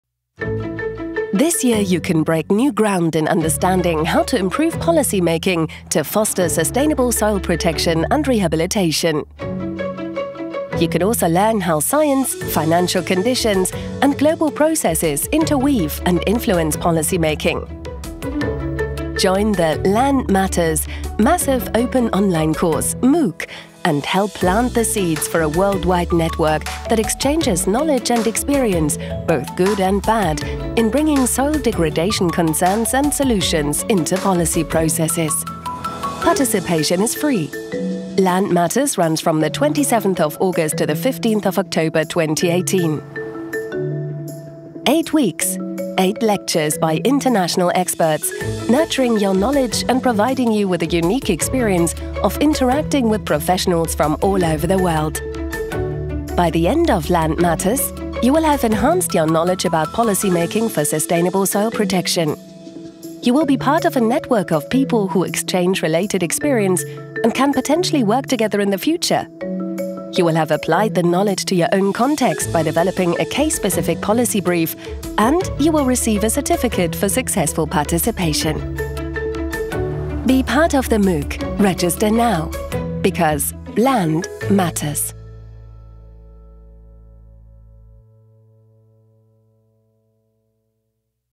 Land Matters! Course Trailer 2018 - English version
Join this Massive Open Online Course (MOOC) implemented by GIZ, the University of Leeds and ICRISAT and
★ Share your experiences in an international network for soil protection!
★ Exchange with experts!
★ Learn how to share your knowledge with those who can influence policies!

The 8-week course, which will run from August 27th through October 15th, 2018, will focus on how to integrate soil degradation concerns and solutions in policy processes as one major aspect of sustainable soil protection.
★ 8 weeks with 8 modules and video lectures
★ 3 hours of work-load/week for participants who would like to receive a certificate
★ Held in English with French subtitles
For more details on the course and registration watch the trailer and go